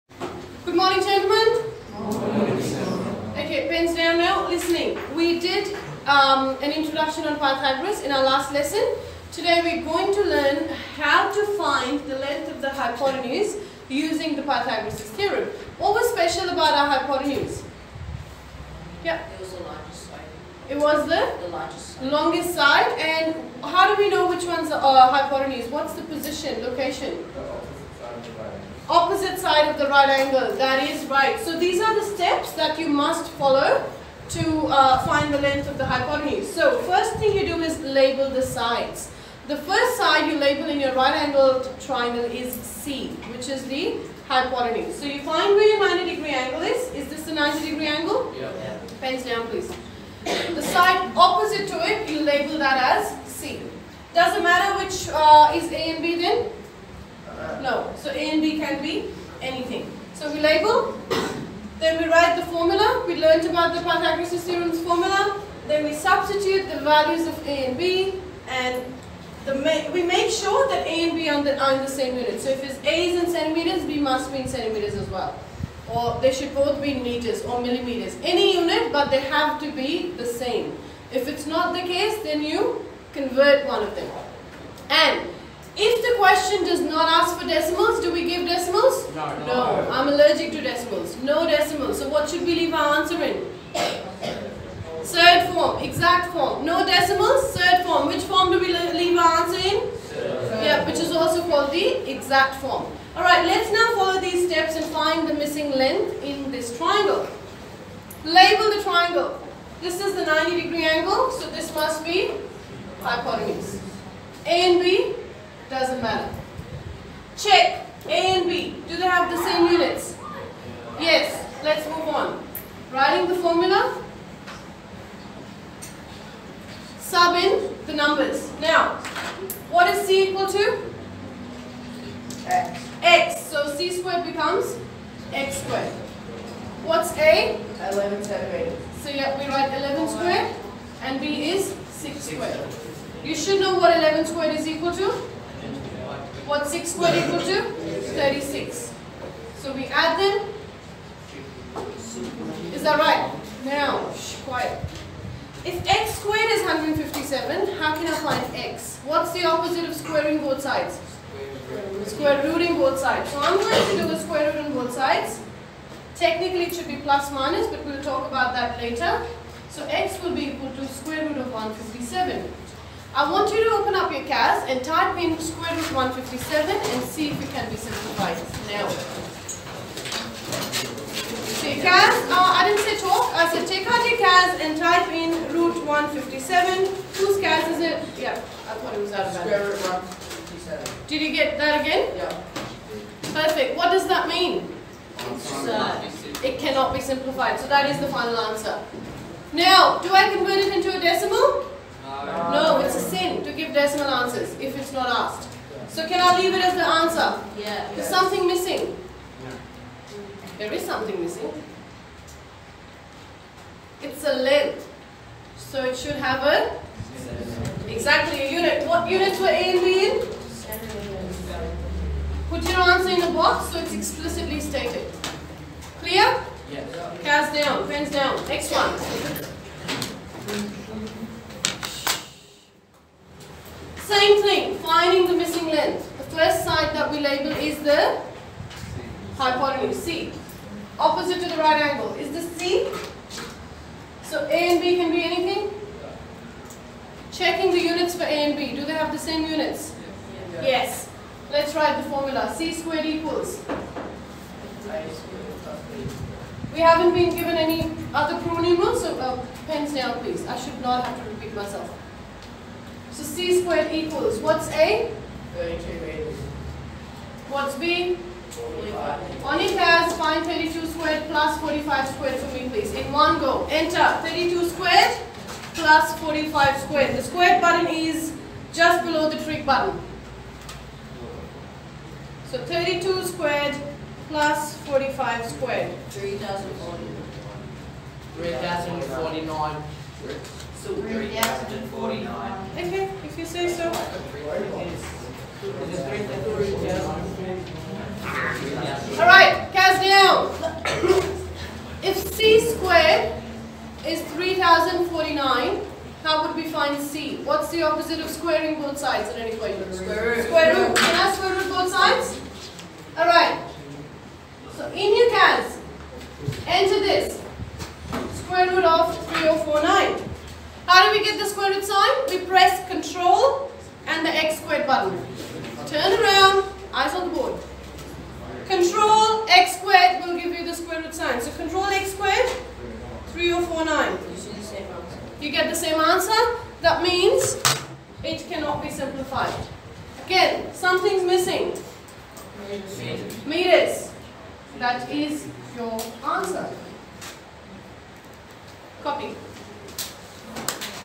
Finding the length of the Hypotenuse using Pythagoras' Theorem made easy!
This video covers the steps for calculating the length of the hypotenuse in a right angled triangle using Pythagoras' theorem.